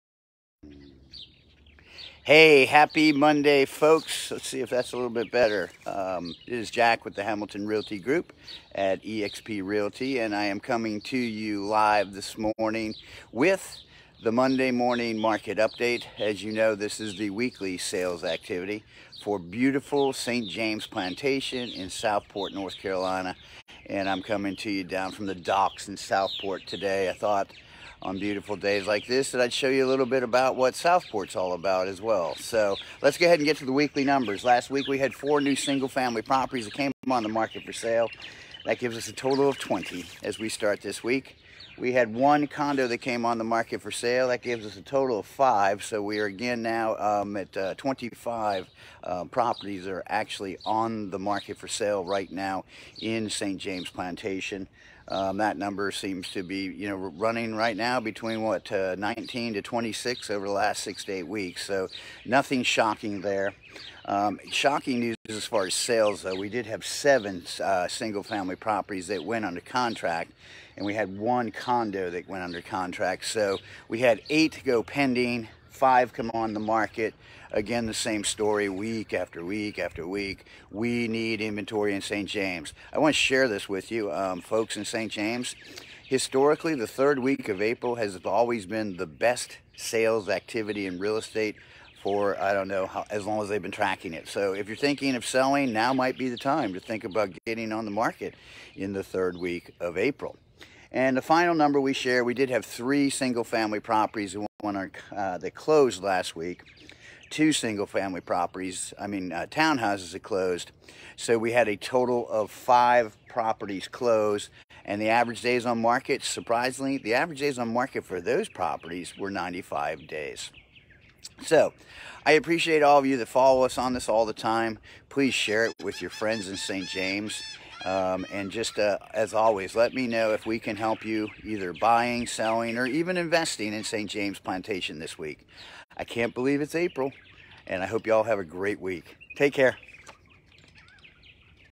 The Monday Morning Market Update - Week Ending 4/2/2023
The Weekly Sales Activity for St. James Plantation in Southport, NC

Hamilton Realty Group
Southport NC Office